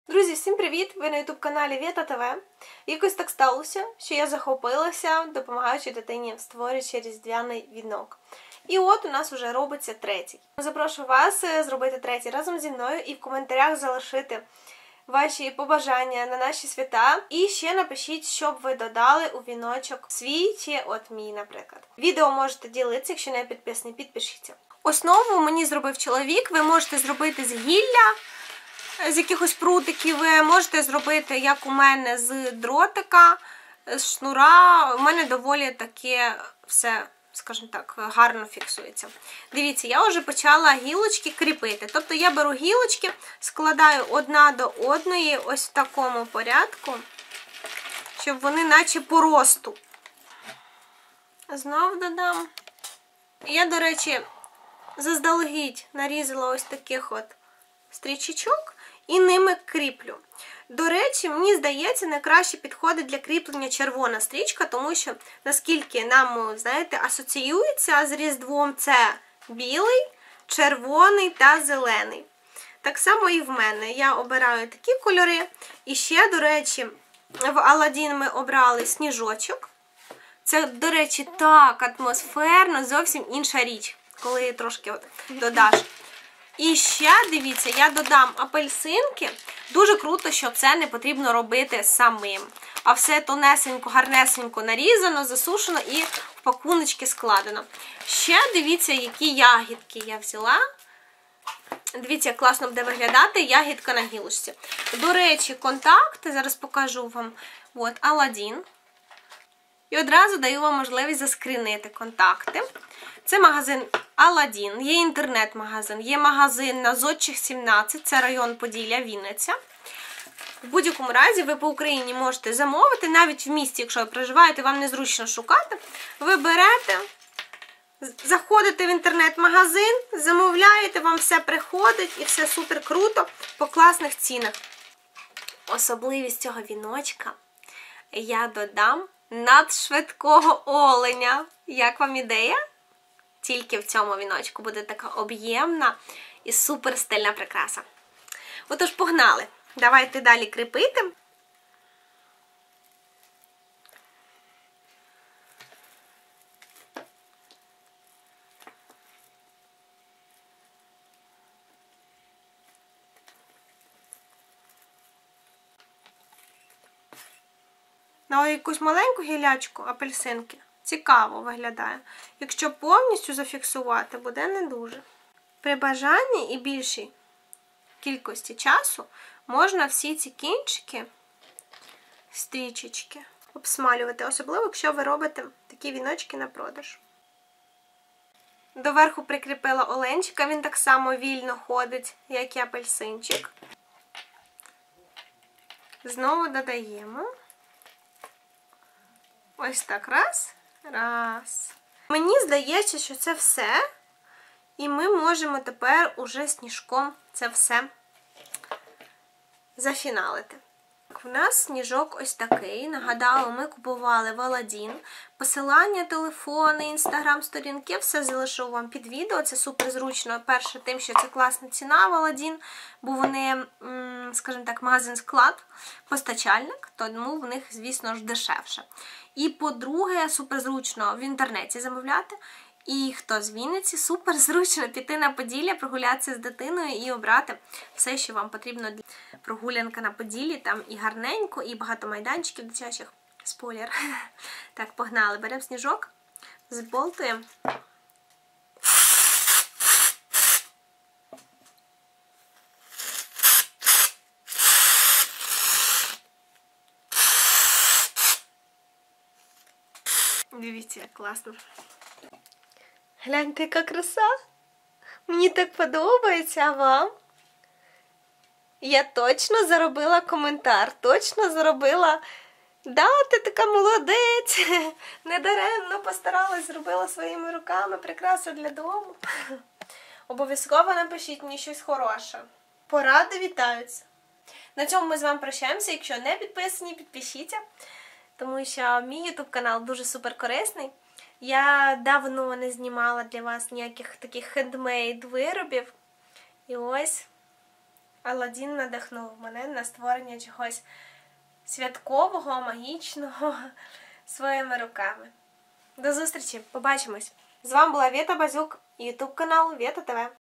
Різдвяний вінок Вєта Базюк
Мені сподобалося допомагати з вінком Лялі і я створила свій. оцініть.
для звязку з магазином матеріалів і подарунків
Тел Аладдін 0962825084
Однокласники: Світлана Базюк
Фейсбук Вета Базюк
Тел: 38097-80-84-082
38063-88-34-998